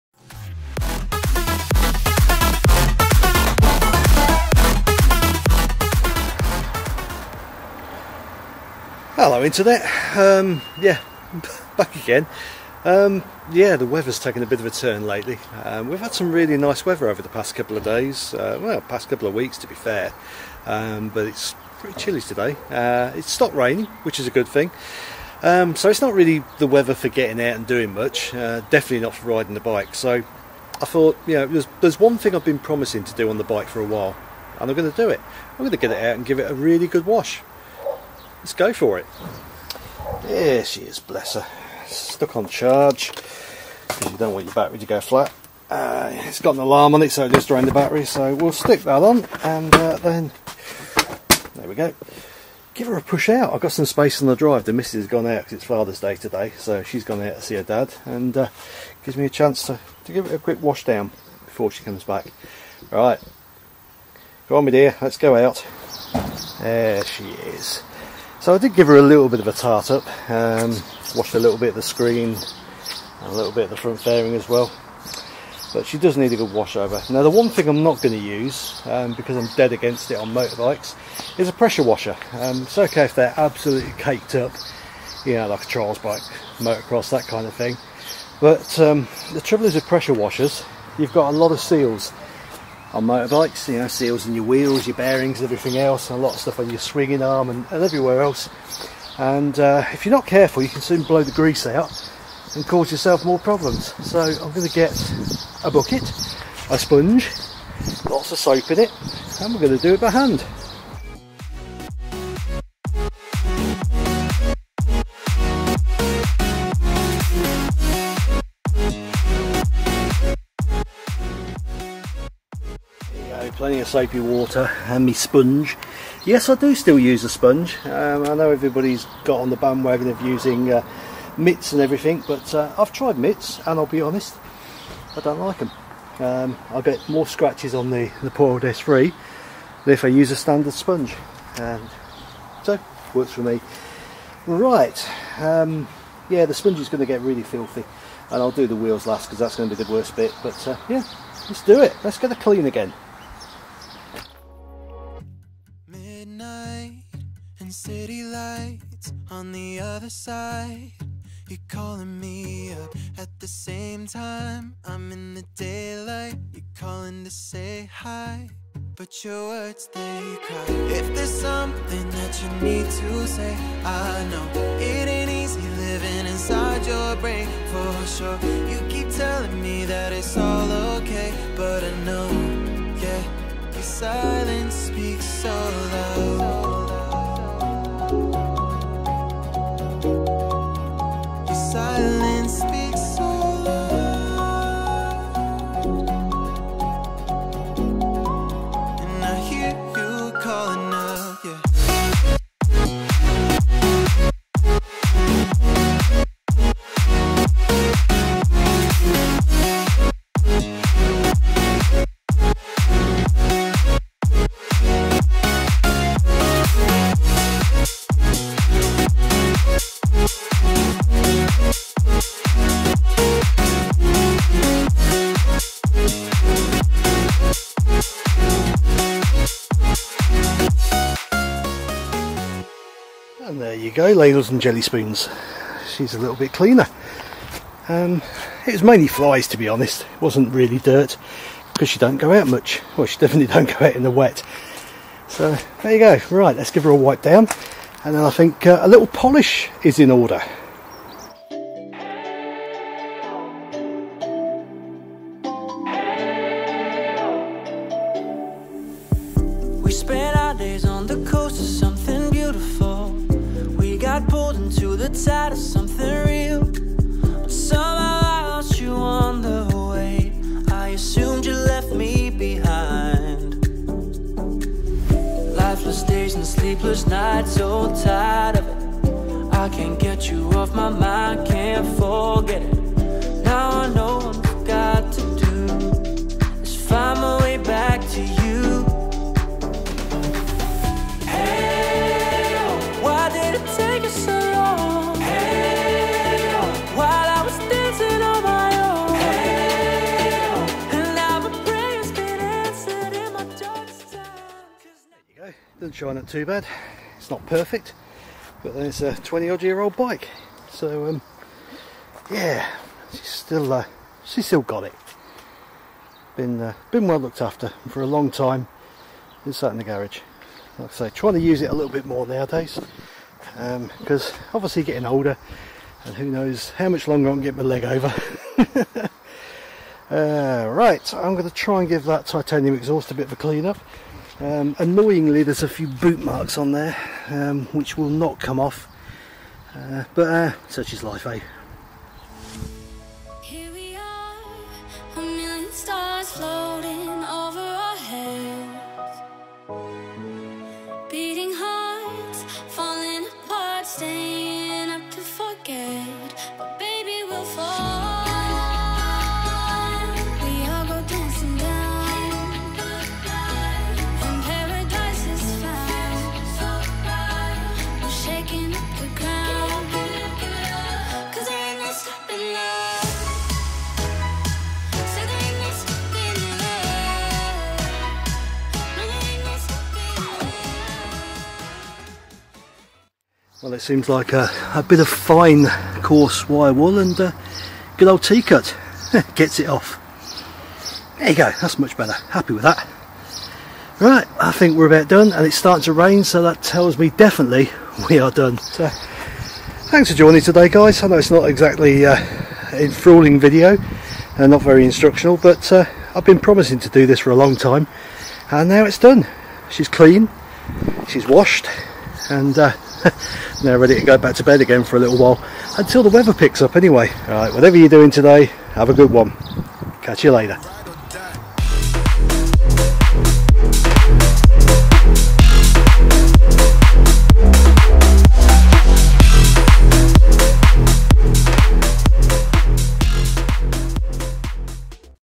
long overdue bath time
Today as the weather is so bad, I did what I have been promising for a long time...Gave the Ninja a well overdue bath...